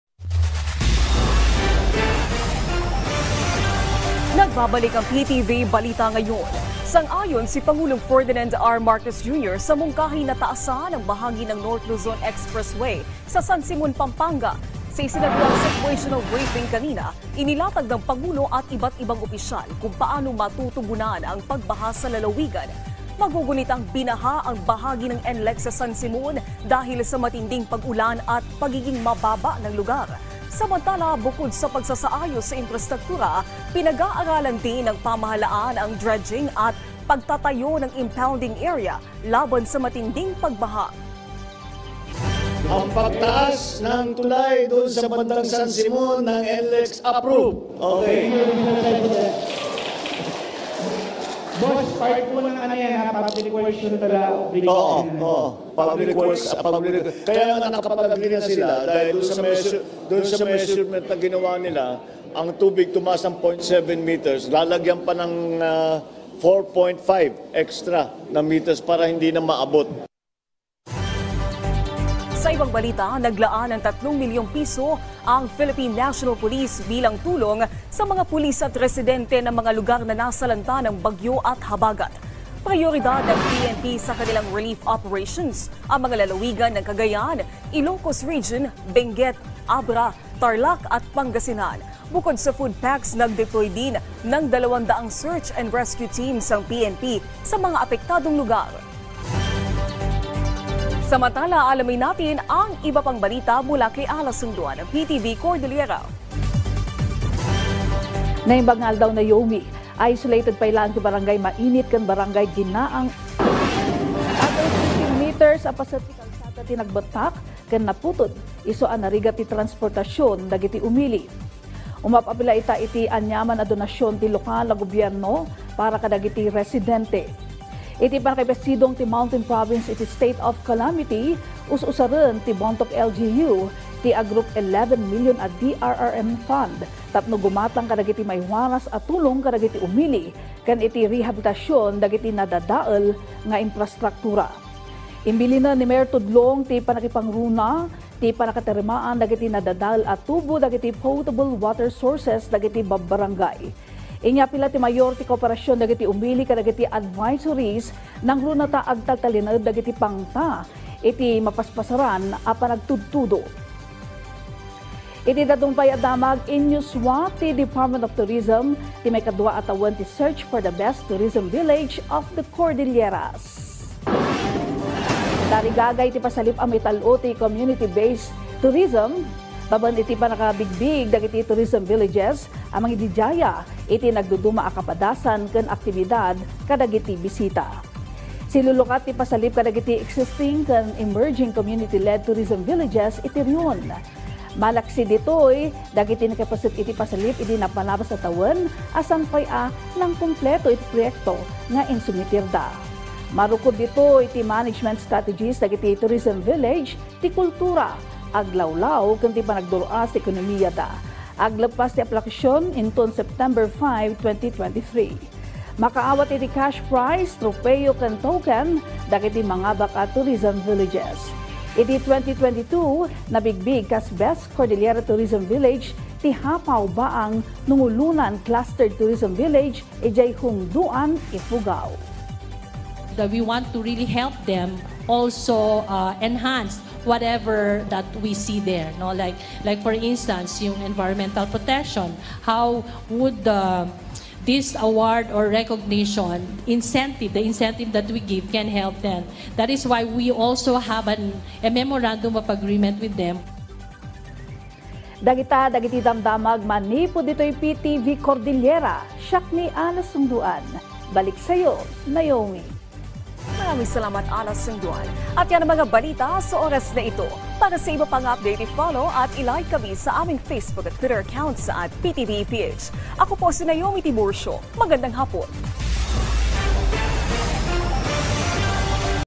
PBBM, sang-ayon sa mungkahi na taasan ang bahagi ng NLEX sa San Simon upang maiwasan ang pagbaha
PNP, naglaan ng P3M para sa relief assistance ng mga naapektuhan ng Bagyong